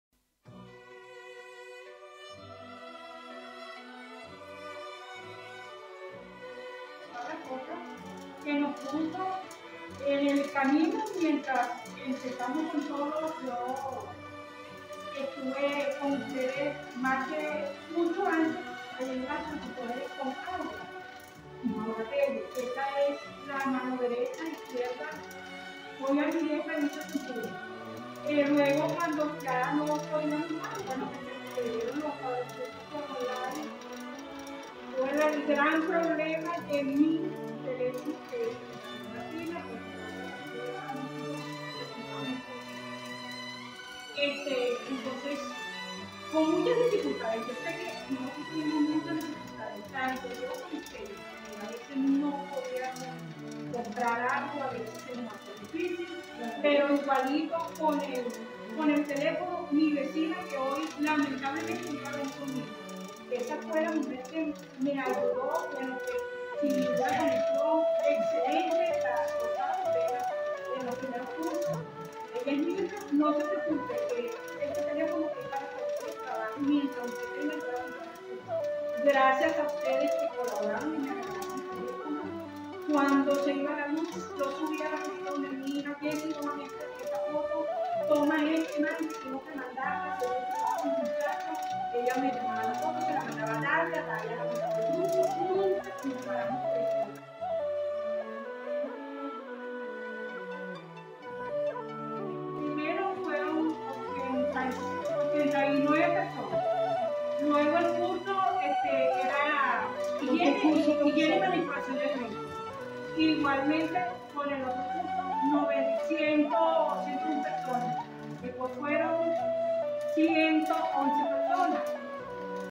La experiencia de nuestros MTP
Esta es tan sólo una de miles de historias... pero se acerca por mucho a la realidad vivida desde todos nuestros CFS. Nuestro reconocimiento y felicitaciones atan ardua labor.